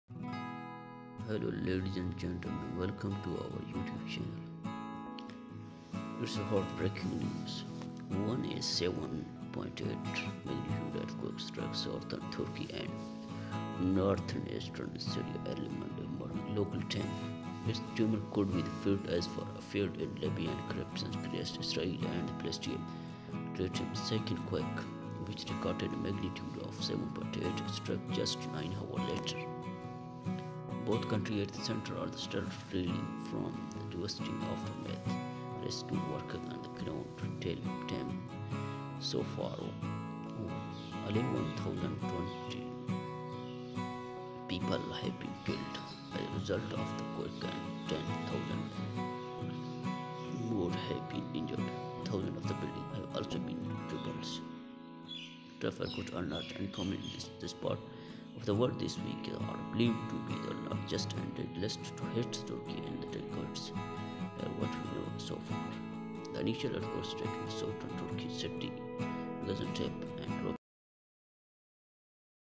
Over 11200 killed after powerful 7.8 earthquake rocks Turkey and Syria, hundreds more trapped
A powerful 7.8 magnitude earthquake rocked wide swaths of Turkey and Syria early Monday, toppling hundreds of buildings and killing more than 11200 people. Hundreds were still believed to be trapped under rubble, and the toll was expected to rise as rescue workers searched mounds of wreckage in cities and towns across the area. STO